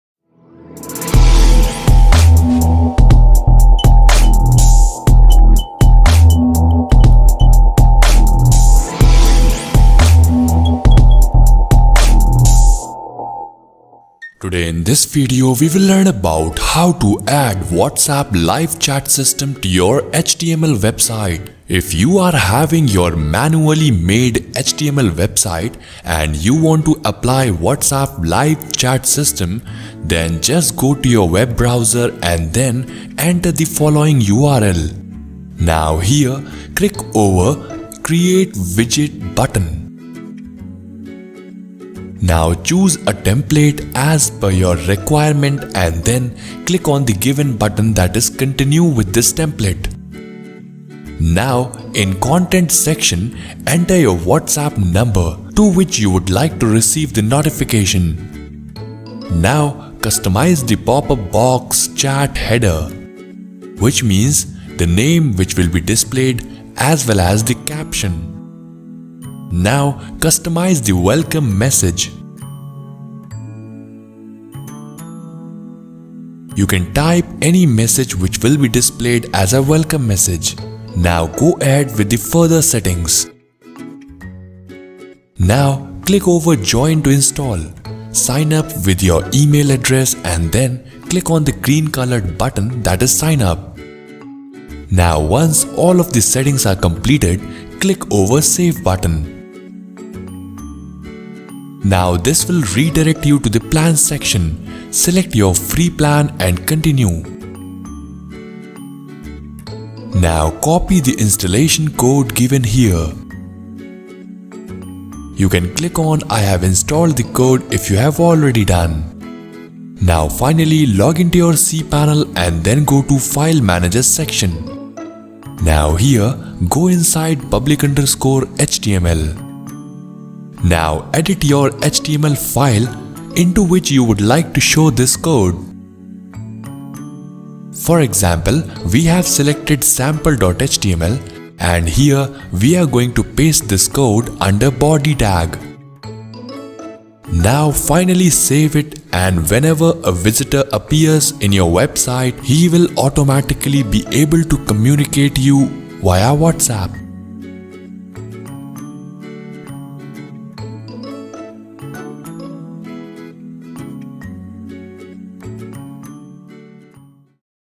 How to Add Whatsapp live chat code to your HTML website?
Today in this video we will show you how to add whatsapp live chat widget to your HTML website. So if you want to apply whatsapp live chat widget in your manually built HTML website then simply follow the steps given below:
Step::2 Click over the Create Widget button
Step::3 Now choose the template as per your choice and click on Continue button
Step::4 Next in the Content section enter the whatsapp number to which you want to receive the notifications
Step::5 Now customize the pop-up box chat header
Step::6 Next customize the welcome message
Step::7 Once the settings are configured click on Join to Install button
Step::8 Now complete the signup process
Step::9 Once all the process is completed, click on Save button.
Step::10 Next you will be redirected to a page with various plans, you can choose any plan as per your need and then click on continue.
Step::11 Copy the installation code
Step::12 Login to your cPanel and click on File Manager
Step::13 Click over public_html
Step::14 Right click over the HTML file and choose the Edit option
Step::15 Now paste the code here under the body tag and click on Save changes button